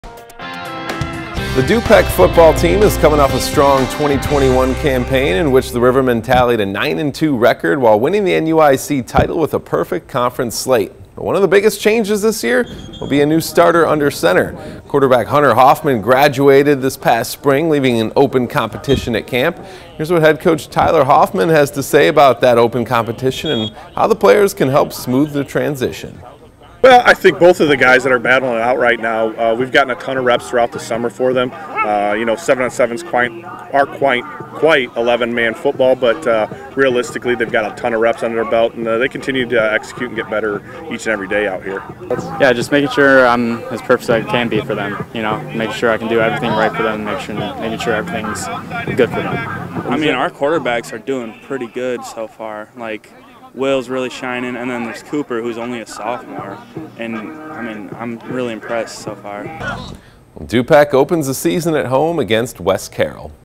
Defending champ Du-Pec looks to watch NUIC throne
Du-Pec looking to replace Hunter Hoffman at QB.